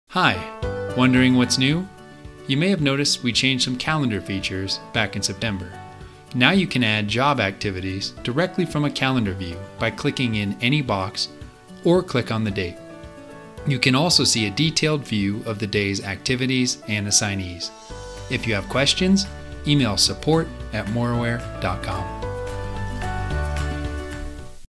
What's New Calendar Updates
Now Systemize Job Activities can be added directly from a Calendar View. Daily details of Activities and Assignees are also accessible in any Systemize Calendar View.